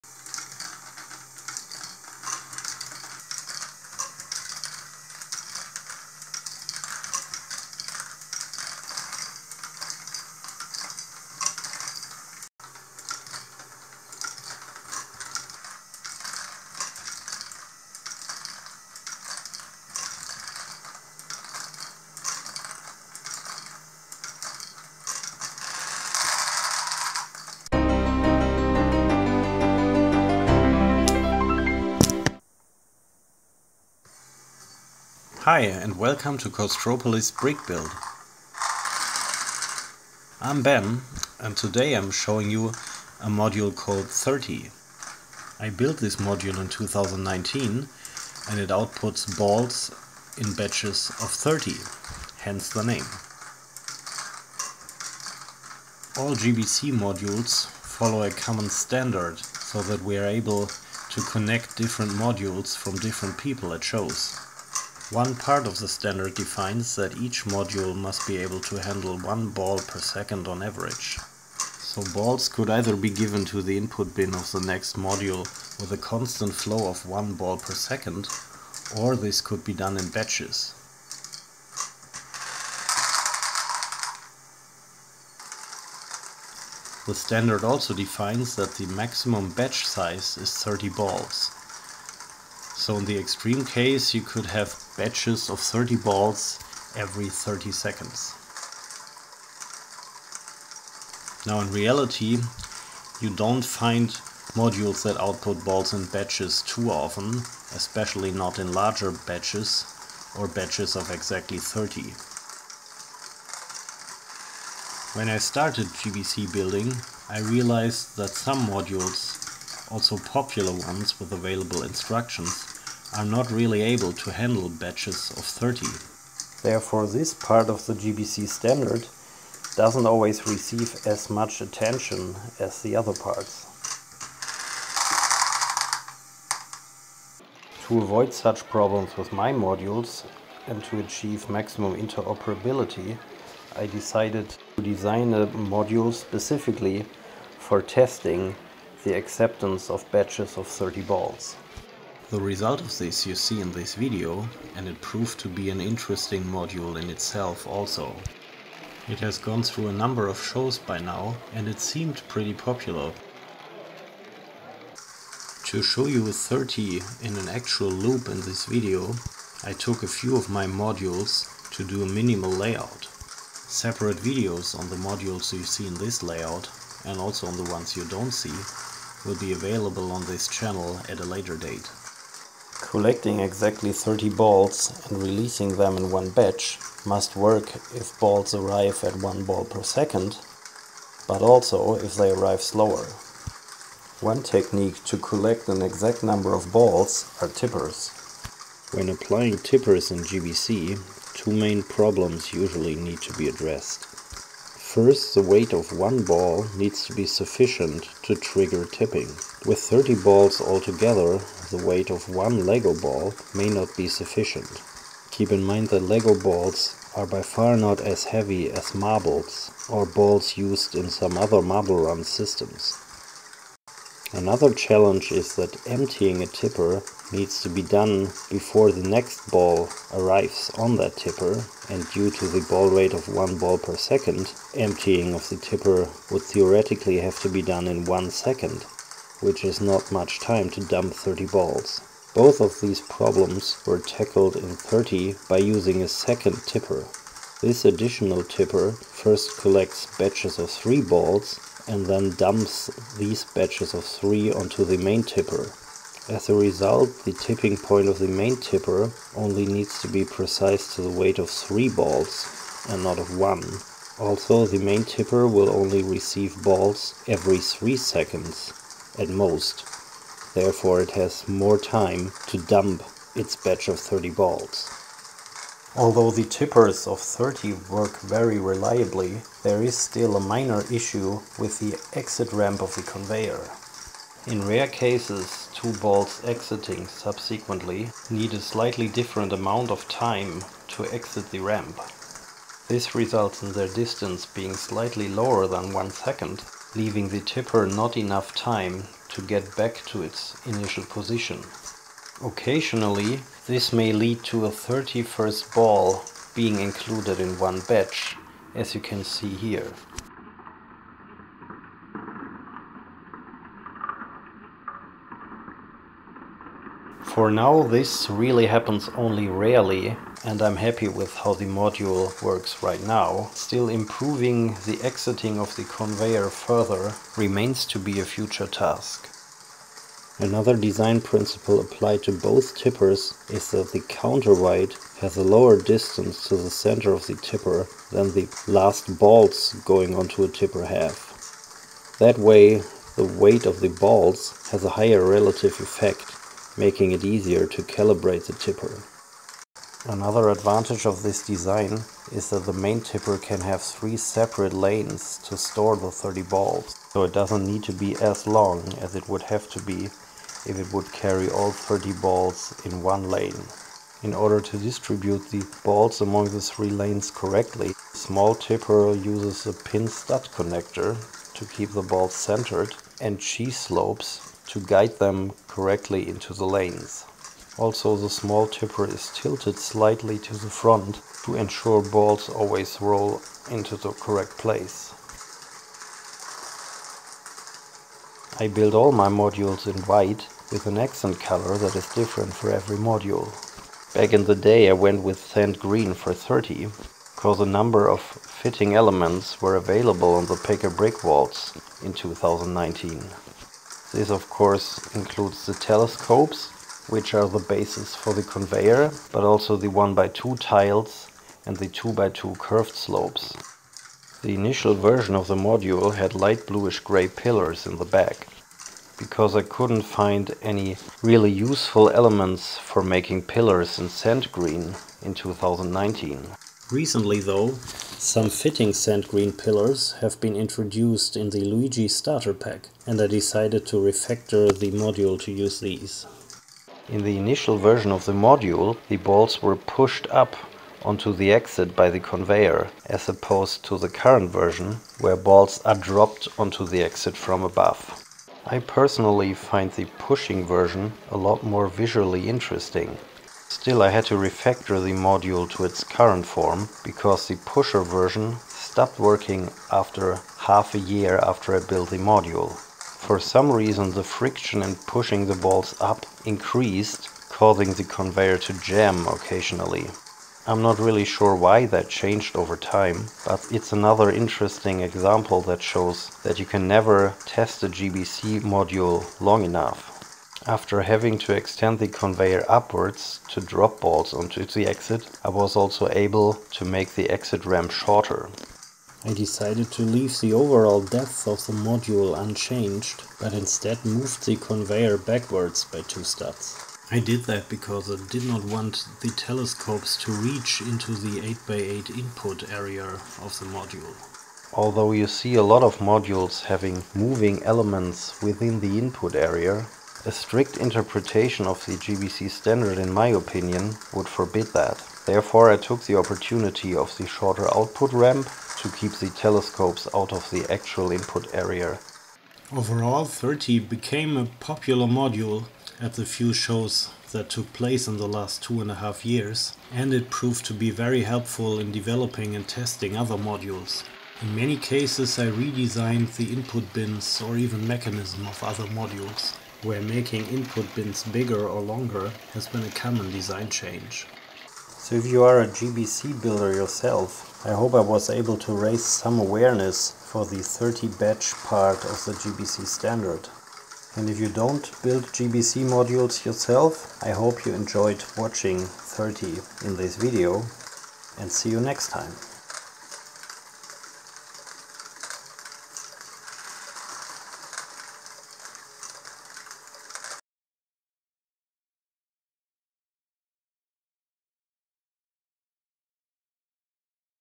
30 - A LEGO GBC Module to test Compliance with the Standard
This GBC (Great Ball Contraption) module collects 30 balls and releases these in one batch into the next module. It's fun to watch and important in testing other modules because accepting batches of 30 balls at once is part of the GBC standard that allows us to combine modules from different builders at shows and create large layouts.

The module is built from 1201 parts with the accent color sand green.